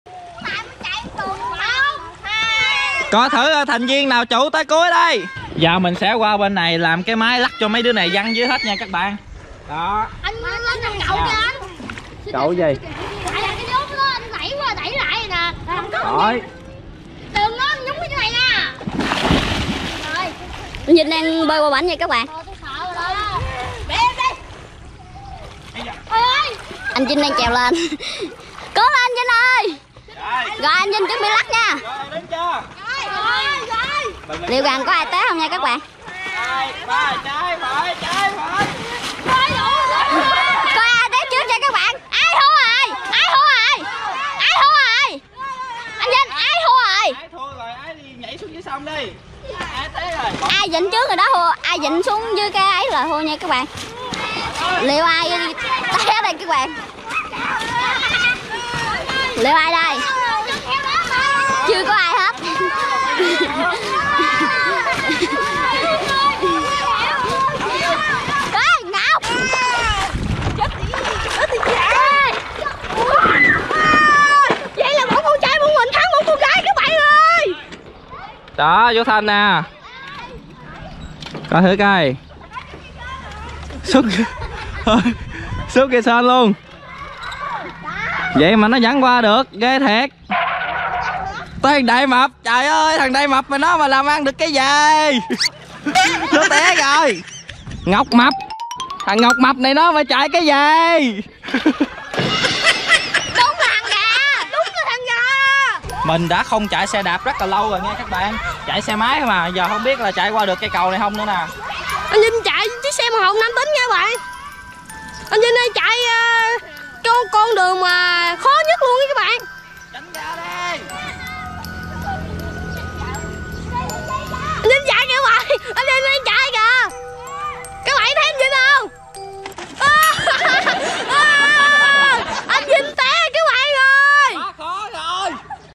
Tony Lái Xe Đạp Qua Sông Cực Đỉnh
Lần đầu chạy xe đạp qua sông, đỉnh kou của biker.

© Bản quyền thuộc về Tony TV
© Copyright By Tony TV & Do not Reup